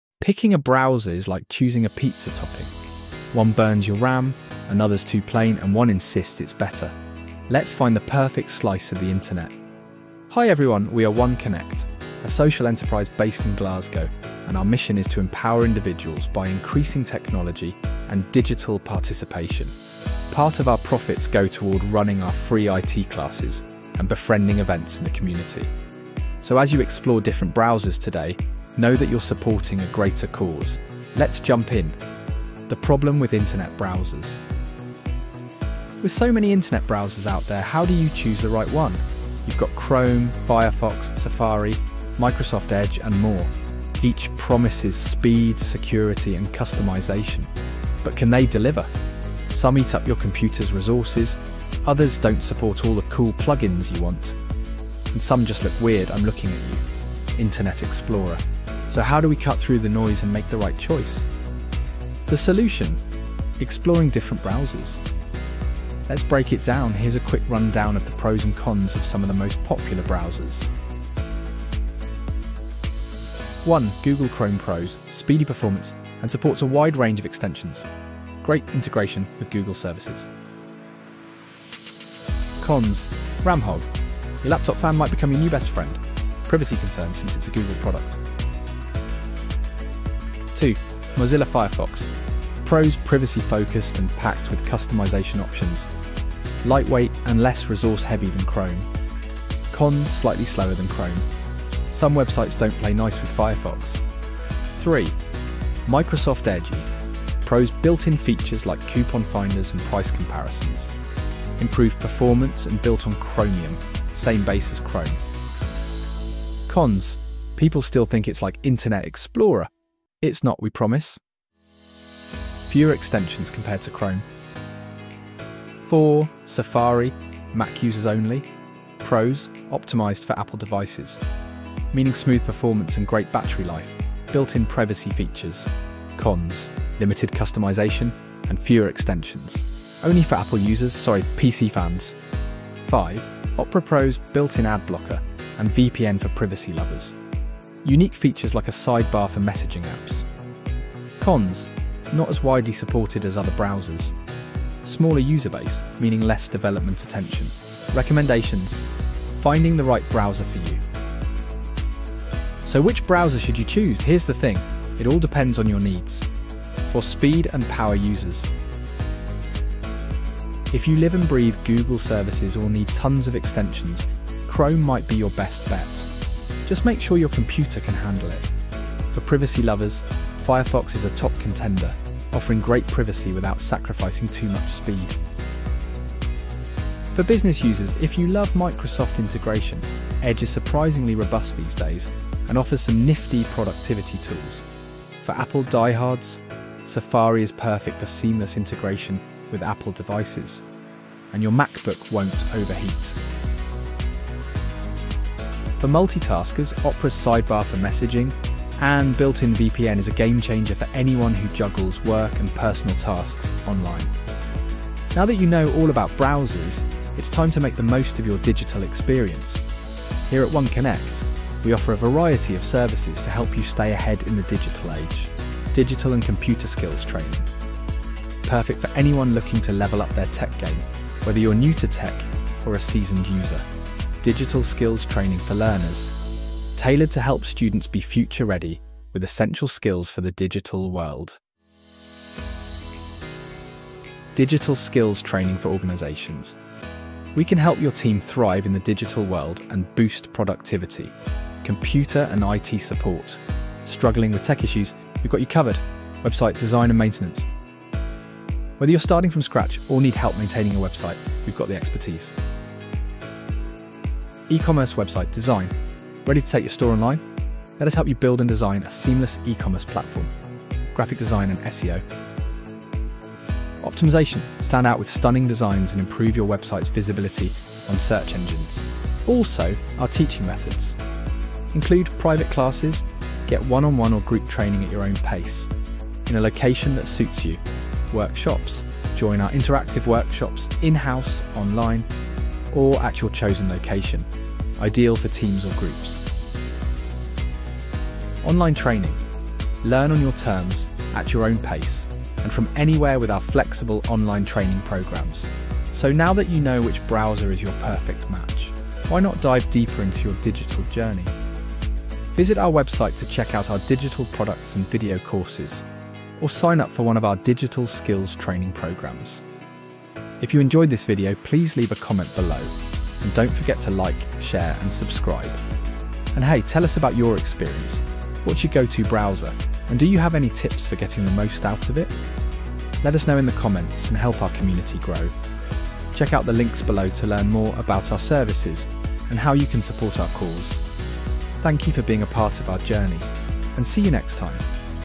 Battle of the Browsers: Which Internet Browser is Your Perfect Match?
Struggling to pick the best internet browser? 🍕 Whether Chrome is eating all your RAM or Firefox has the features you need, we’ve got you covered! In this video, we’ll compare the pros and cons of Chrome, Firefox, Safari, Edge, and more! 💥

Find out which browser is the best fit for speed, security, and customization—all while supporting Won Connect, a social enterprise empowering people with digital skills. 🚀

YOUR PURCHASE ALLOWS US TO RUN FREE EVENTS AND COMPUTER CLASSES FOR THE ELDERLY COMMUNITY. THANK YOU!